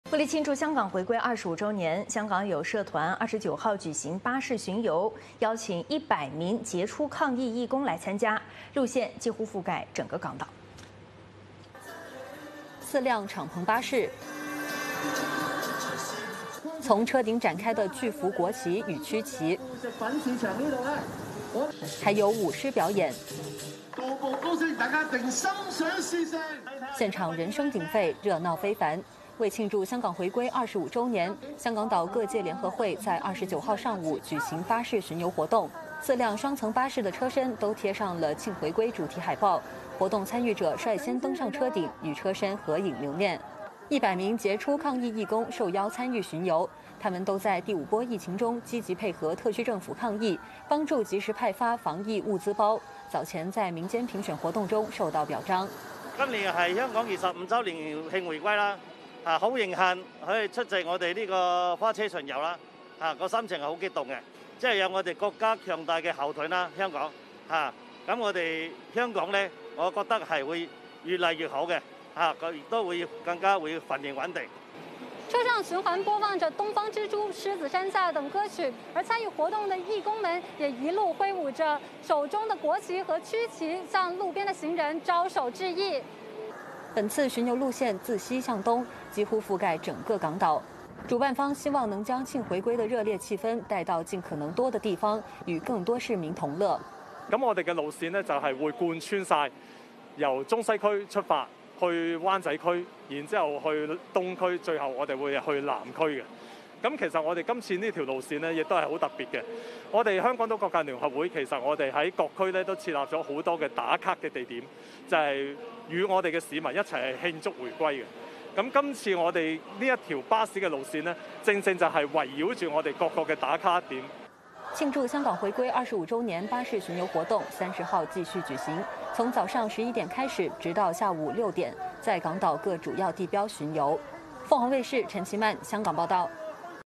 香港團體舉辦慶回歸巴士巡遊 路線覆蓋全港島
香港團體舉辦巴士巡遊活動以慶祝香港回歸25周年，有100名優秀抗疫義工受邀參加活動。巡遊路線貫穿全港島，從中西區出發，經灣仔去到東區、南區。本活動還包括在各區的社區設立多處打卡點，與市民一起慶祝香港回歸。